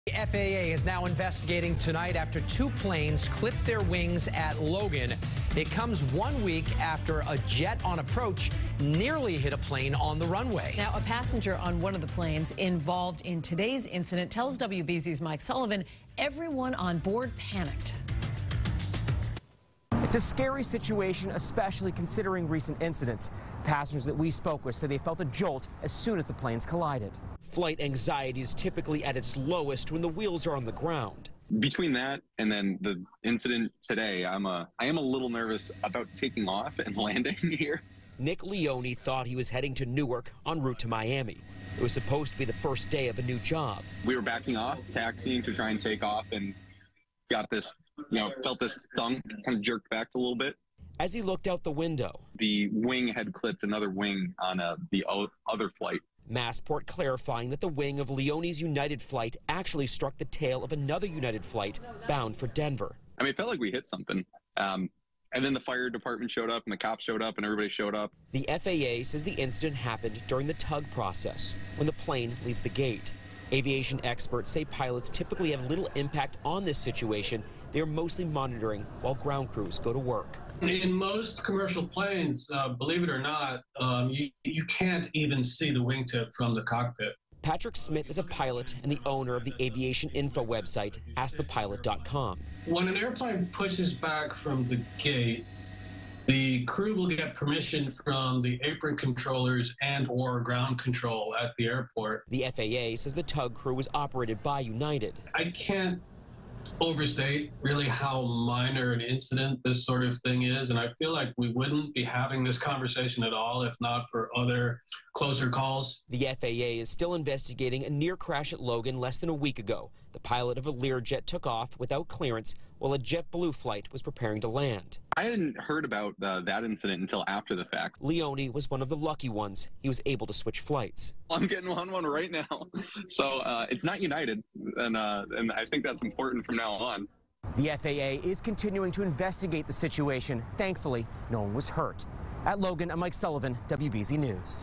United Airlines planes clip wings at Logan Airport
Two United Airlines jets clipped wings at Logan Airport Monday. WBZ-TV's Mike Sullivan reports.